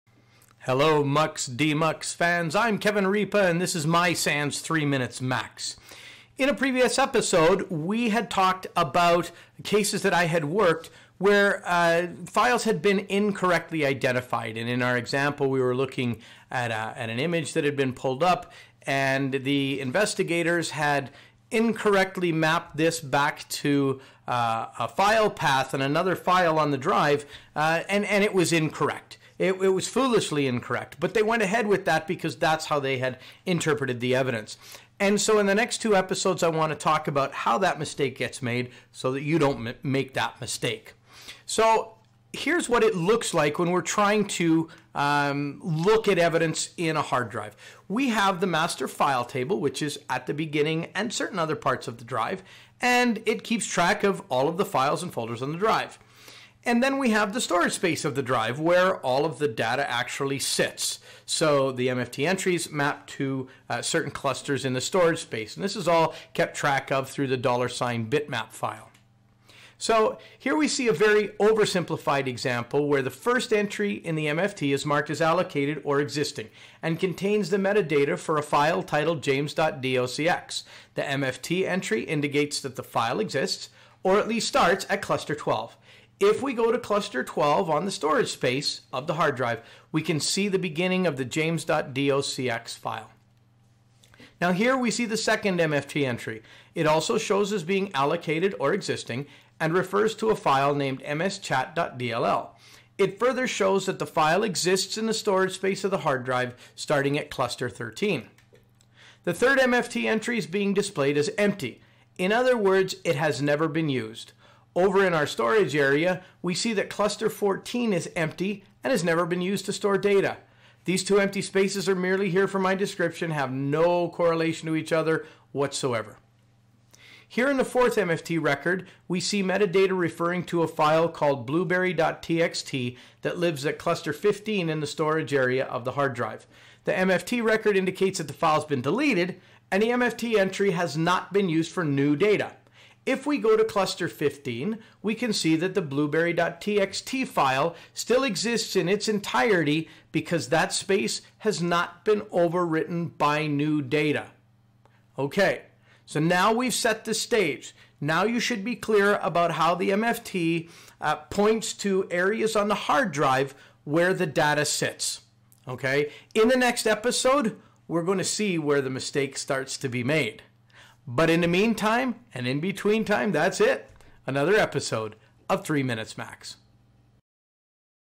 Episode 92: Case study on an incorrect conclusion in a criminal case-Part 2
In this episode, we describe the correlation between the MFT and the data it points to. More importantly, what happens between them during file deletion and subsequent data space reuse. This sets the groundwork for helping us understand how the mistakes got made regarding our case study described in Part 1 of this series.